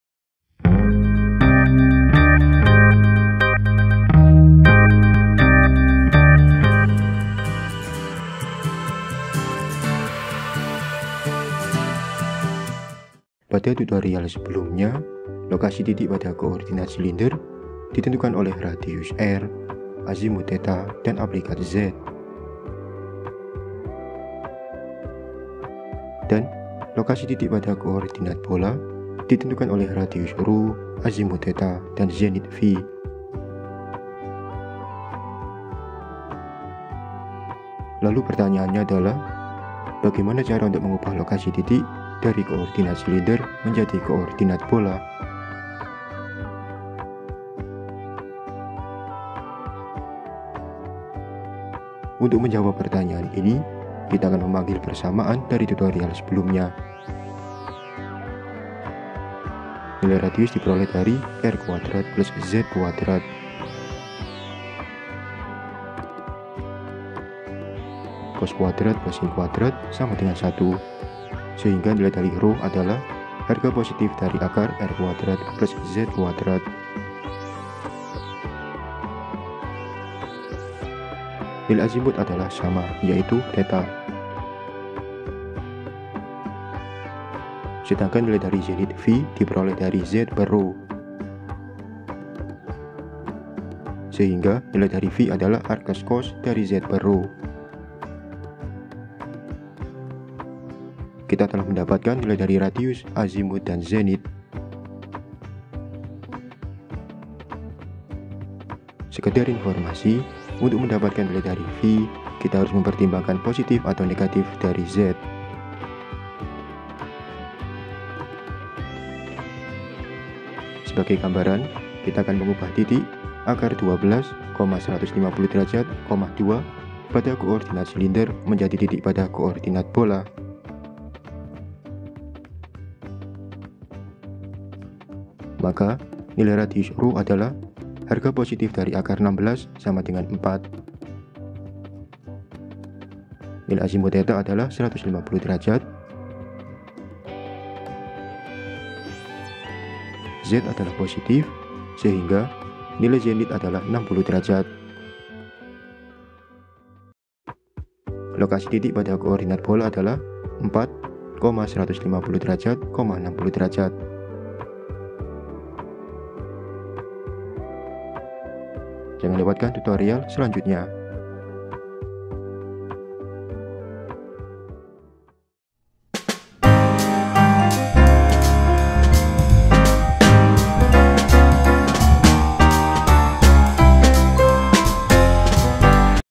Bagaimana mengubah koordinat tabung menjadi koordinat bola pada titik
Lokasi titik pada koordinat tabung (cylindrical) ditentukan oleh radius r, azimuth θ, dan aplikat z. Sedangkan lokasi titik pada koordinat bola (spherical) ditentukan oleh radius ρ, azimuth θ, dan zenith φ.

Pada tutorial ini, kita akan merubah lokasi titik  dari koordinat tabung menjadi titik pada koordinat bola. Akan diperoleh konversi sebagai berikut:

Nilai dari radius, ρ adalah √(r²+z²)
Nilai dari azimuth, θ adalah θ
Nilai dari zenith, φ adalah arc cos (z/ρ)

Setelah itu, kita akan menguji pada titik tertentu sebagai ilustrasi